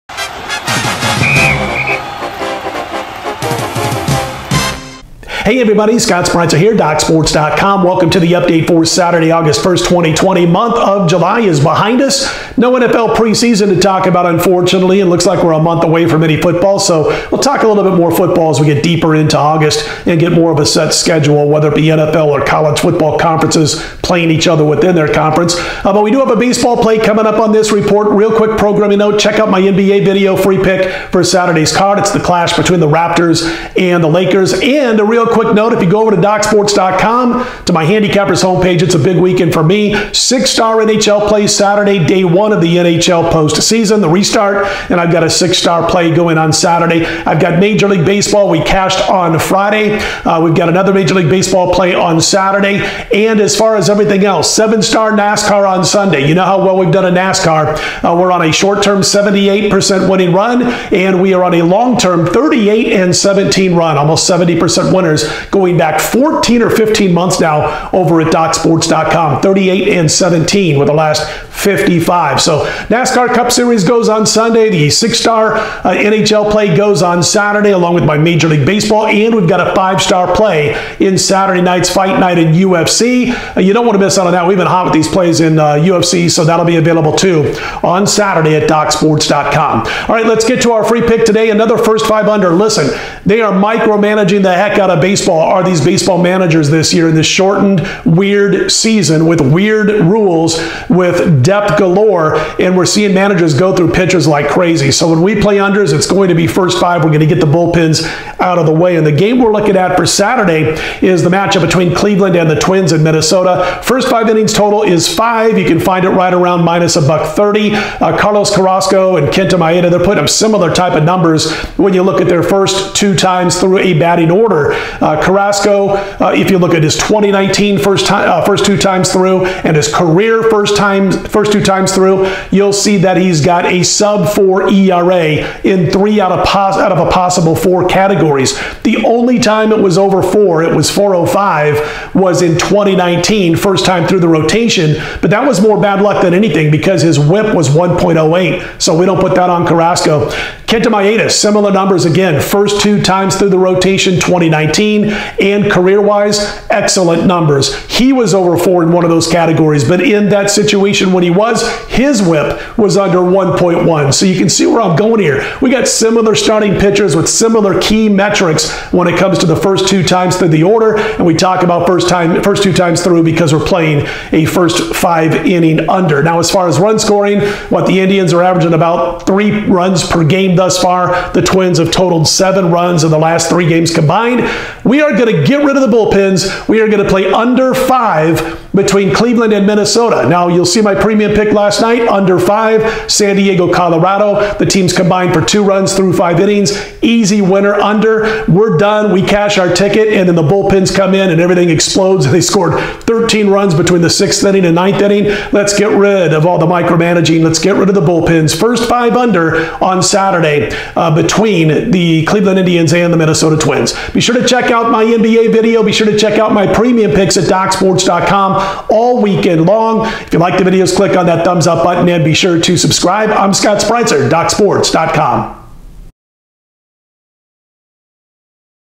Cleveland Indians vs Minnesota Twins MLB picks, predictions tips and odds 8/1/20
Minnesota Twins vs Cleveland Indians MLB picks and predictions 8/1/20. The Cleveland Indians travel to Minneapolis, MN to face the Minnesota Twins at 7:10PM EST at Target Field. Doc's Sports provides MLB picks, predictions, tips and MLB odds on this matchup of Cleveland Indians vs Minnesota Twins